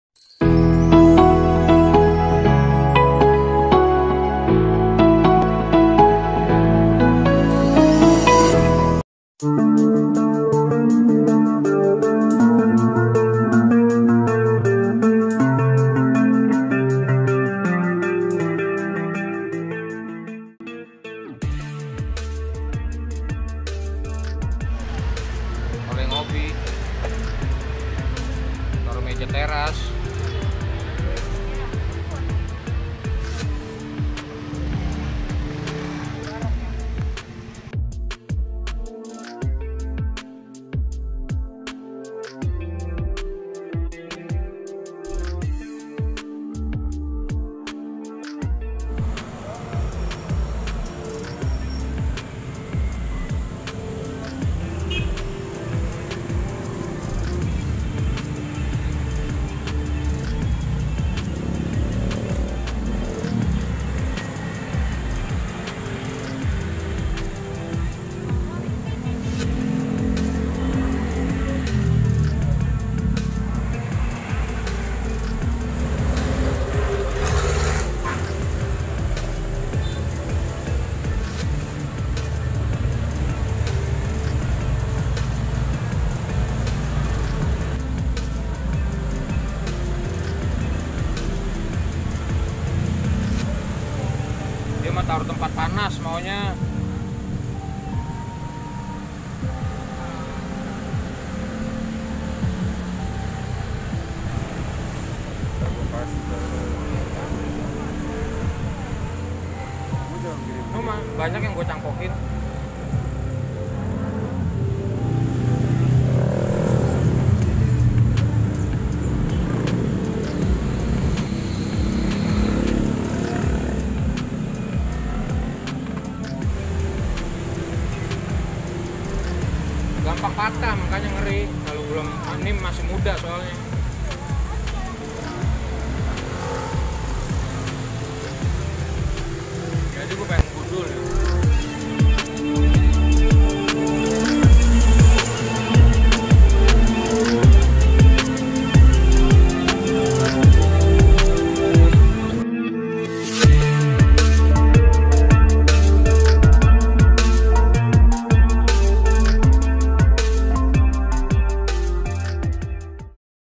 REVIEW BONSAI SAENG SIMBUR MIKRO
Hi sobat, berikut ini adalah video singkat tengtang tanaman Bahan Bonsai Saeng simbur mikro, ukuran daun bonsai Saeng simbur mikro kecil, dan untuk batang Saeng simbur mikro sangat lentur, bonsai saeng simbur mikro bisa jadi inspirasi untuk mwmbua